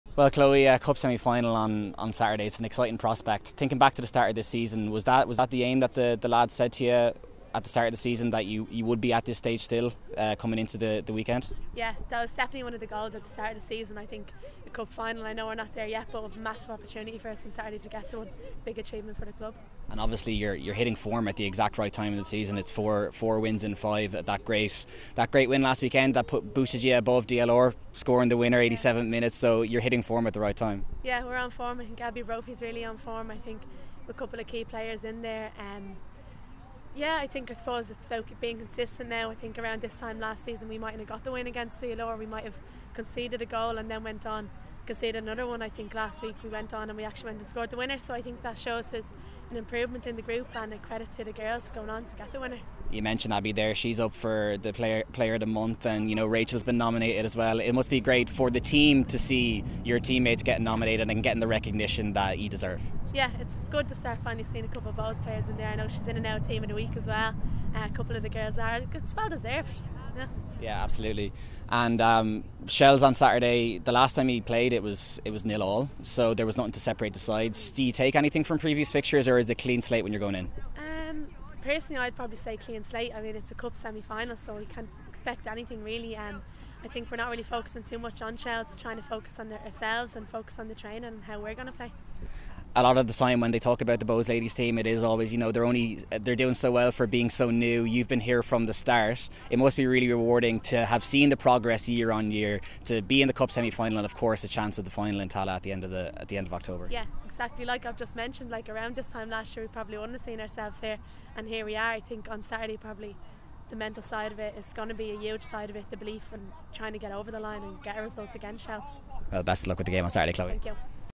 Preview v Shelbourne: Chloe Darby
🏆 Bohemians travel to Tolka Park tomorrow to face Shelbourne in the semi-finals of the Evoke.ie FAI Women's Cup.

🎙️ Chloe Darby spoke to Colm McDonnell ahead of the game.